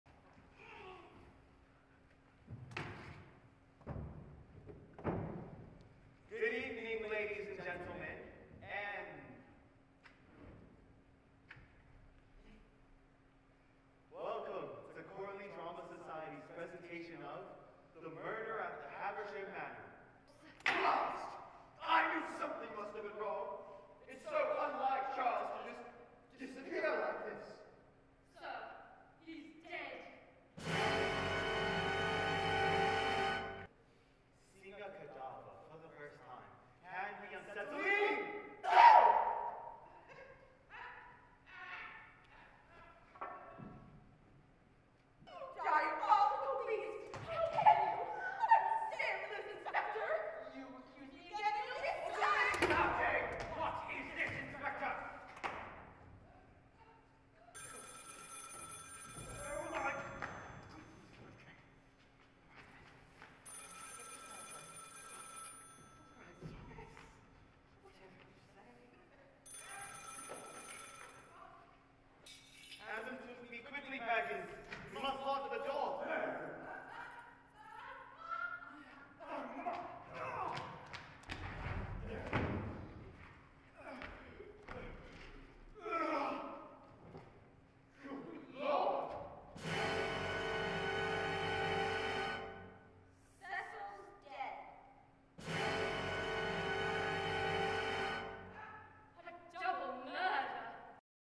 The Loft Stage presents The Play That Goes Wrong
“Break a leg!” takes on a whole new meaning for a woefully misguided troupe of players at the Cornley University Society’s opening night performance of The Murder at Haversham Manor. An unconscious leading lady, a corpse who can’t play dead, a ruffled detective, and a word-mangling butler (among others) must battle against technical gaffes, forgotten lines, and sabotaging scenery in a quest to arrive all in one piece at the final curtain call. Part Monty Python, part Sherlock Holmes, all mayhem, this 1920s whodunit is disastrously delightful.